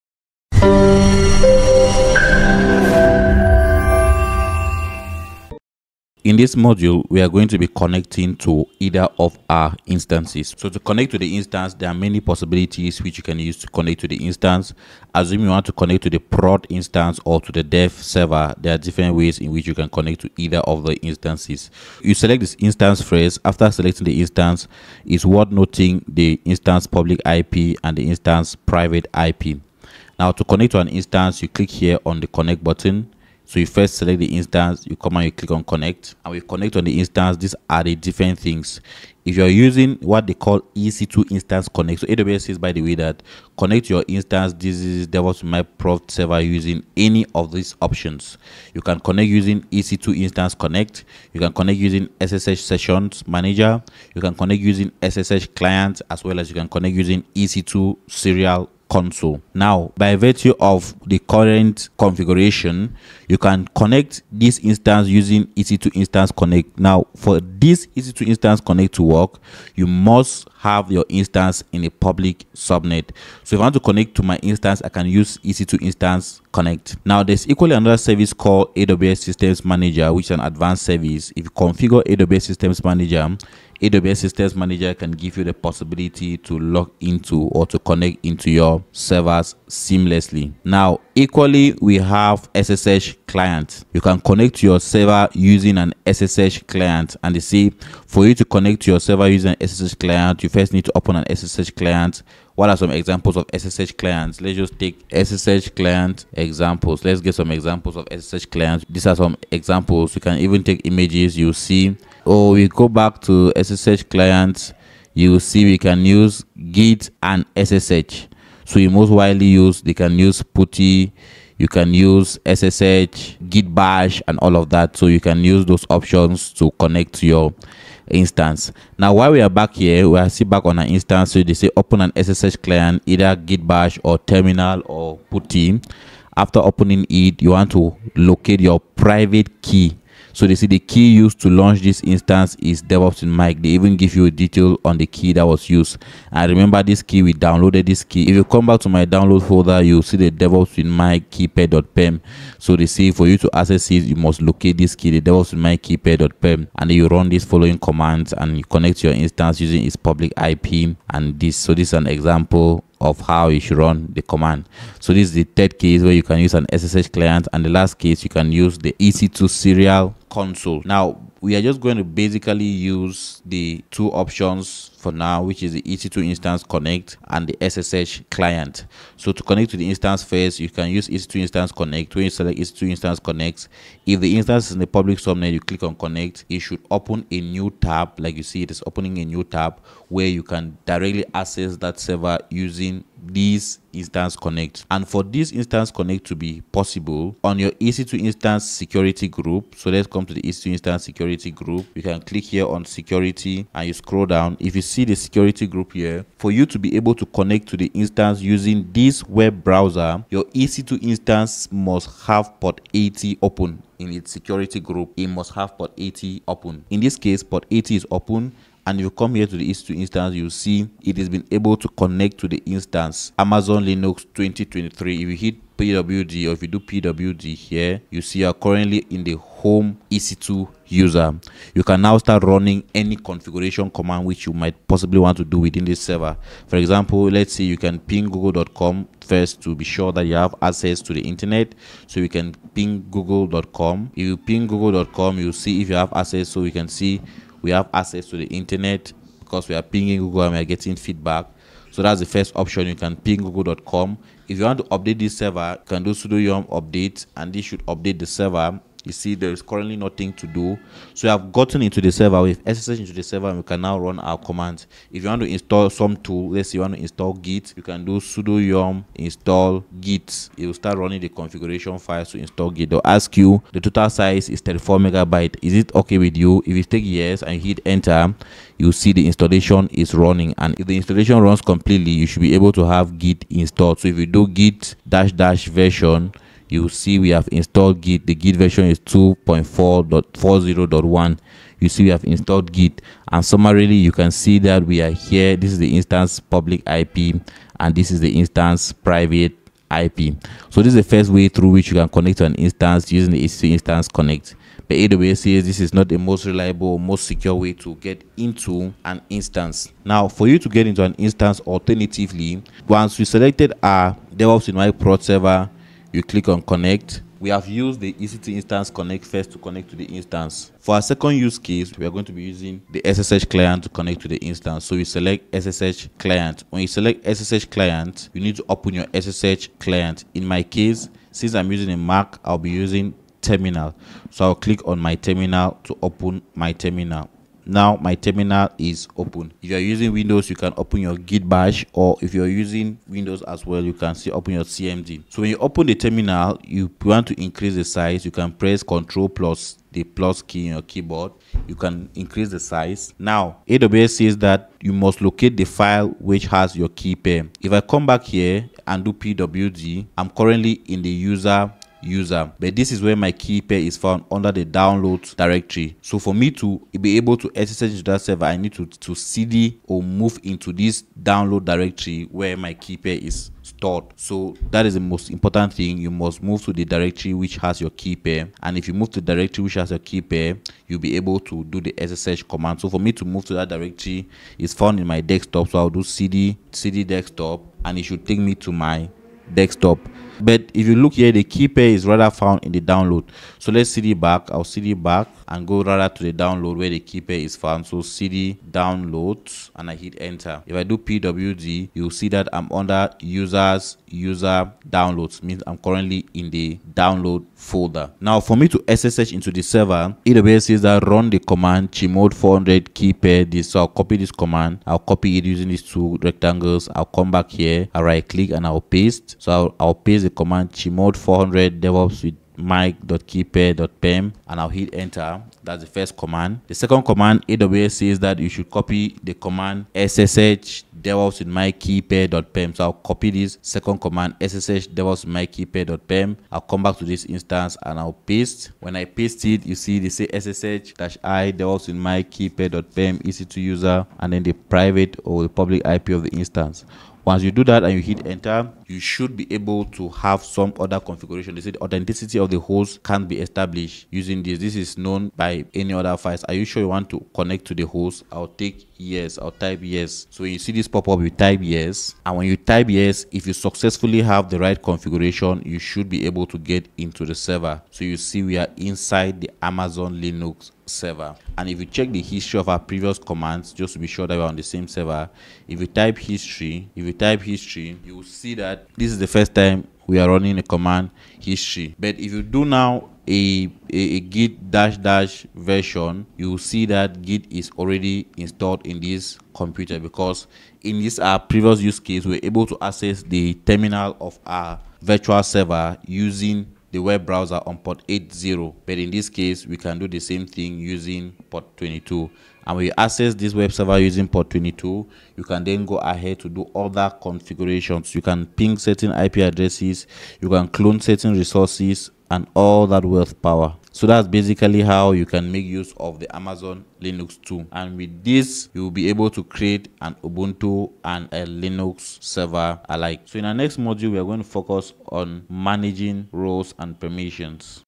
How to SSH into an EC2 Instance | AWS EC2 SSH Access Guide | Step-by-Step | Part 3
In this video, we’ll guide you through the process of SSH-ing into your EC2 instance on AWS. Learn how to securely connect to your EC2 instance using SSH, step by step. This AWS EC2 SSH Access Guide will show you how to set up your SSH keys, connect to your instance, and troubleshoot common issues.

What you'll learn:

✔️ How to SSH into your EC2 instance
✔️ Setting up SSH keys for secure access
✔️ Troubleshooting common SSH connection issues

Perfect for developers and IT professionals looking to securely access and manage their EC2 instances.

All content in this video is owned by the channel owner, Mike.

▬▬▬▬▬ New to “Devops with Mike”? ⬇️  ▬▬▬▬▬